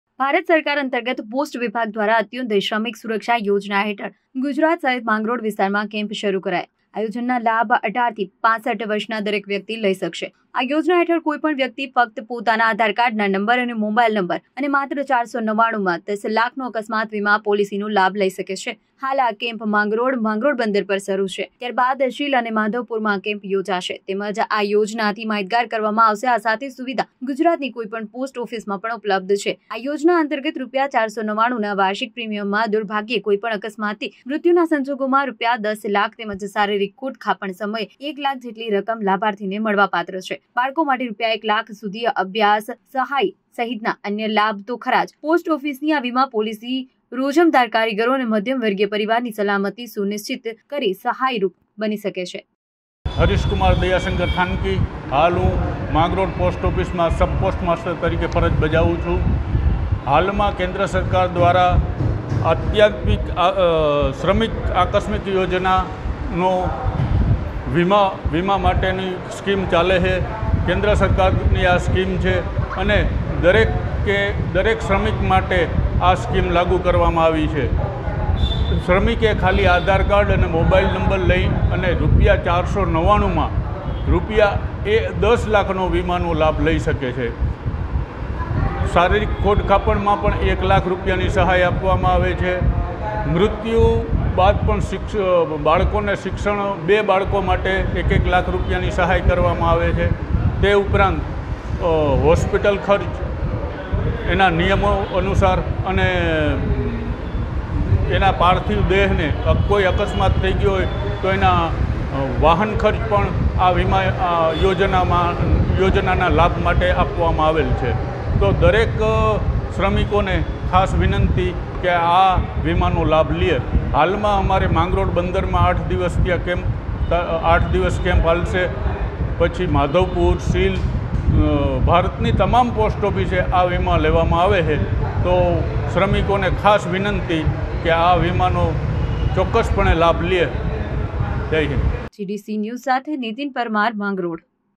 માંગરોળમાં પોસ્ટ ઓફિસ દ્વારા અંત્યોદય શ્રમિક સુરક્ષા યોજના અંતર્ગત અકસ્માત વીમા કવચ કેમ્પનુ આયોજન.
જુનાગઢના "માંગરોળમાં પોસ્ટ ઓફિસ દ્વારા અંત્યોદય શ્રમિક સુરક્ષા યોજના અંતર્ગત અકસ્માત વીમા કવચ કેમ્પનુ આયોજન.